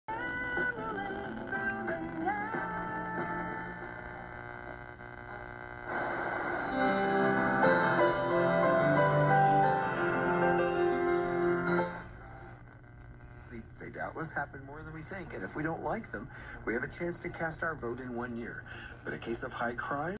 Bendix 526B
Bendix 526B art deco radio from 1947, brought back to life!